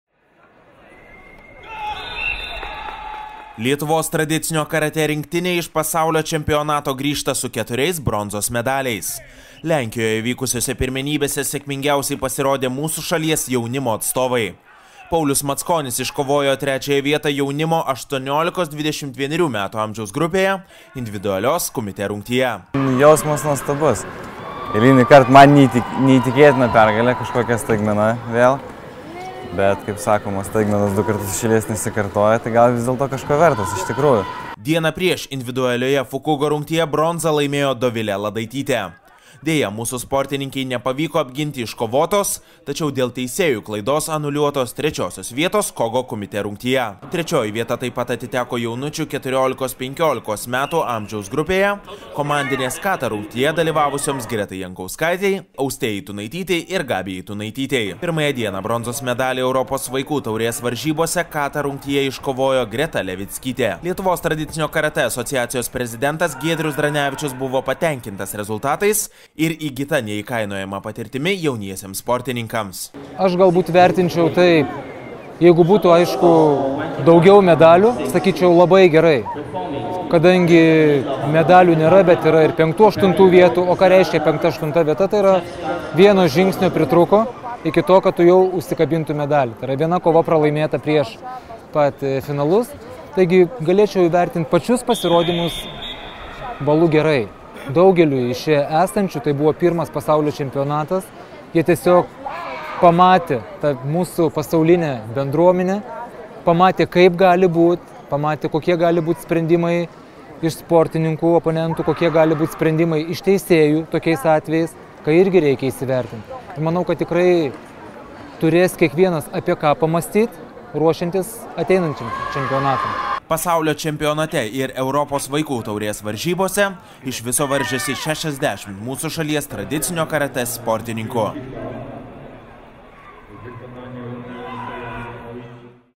Lietuvos tradicinio karatė rinktinei -- keturi bronzos medaliai
Lietuvos tradicinio karatė rinktinė iš Pasaulio čempionato grįžta su keturiais bronzos medaliais. Lodzėje (Lenkija) vykusiose pirmenybėse sėkmingiausiai pasirodė mūsų šalies jaunimo atstovai.  Paulius Mackonis iškovojo trečią vietą jaunimo (18-21 metų) amžiaus grupėje individualios kumite rungtyje. Dieną prieš individualioje fuku-go rungtyje bronzą laimėjo Dovile Aladaitytė. Deja, mūsų sportininkei nepavyko apginti iškovotos, tačiau dėl teisėjų klaidos anuliuotos, trečiosios vietos ko-go kumite rungtyje. Po antrojo bandymo Dovilė užėmė 5-8 vietą. Kata rungtyje sportininkė liko septinta.